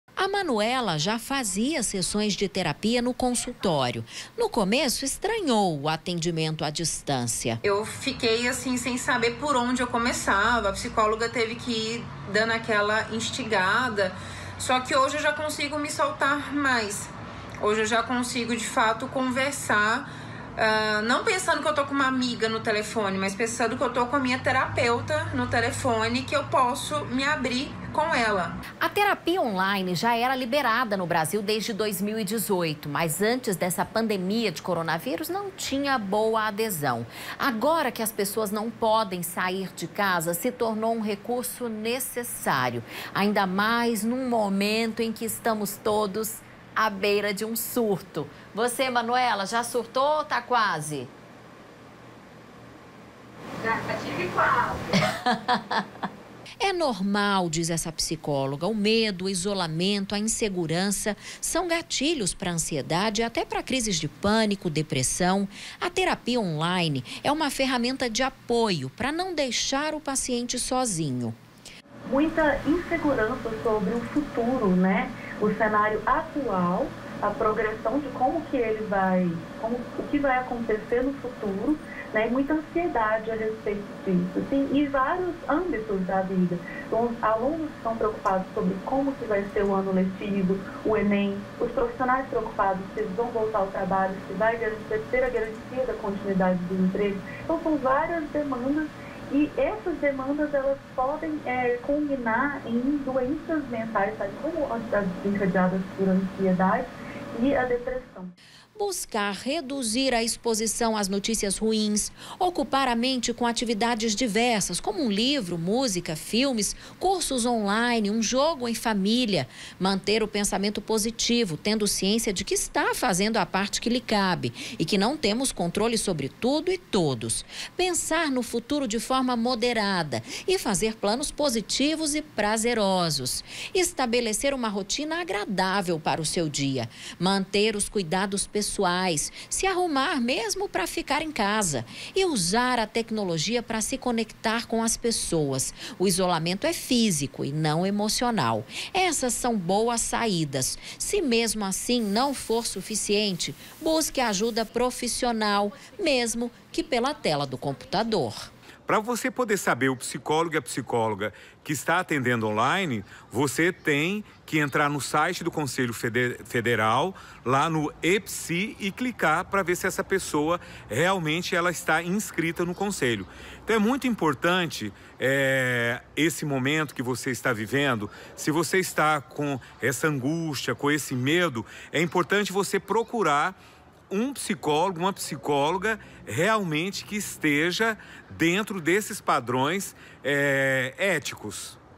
Terapia on-line: alternativa para não surtar durante a quarentena
Do dia pra noite, a gente passou de uma vida frenética para uma quarentena. É um baque psicológico para qualquer um e para não surtar, muita gente precisa da ajuda de psicólogos. Eles nunca atenderam tanto, mesmo pela internet ou por telefone. GoiasRecord @RecordTVGoias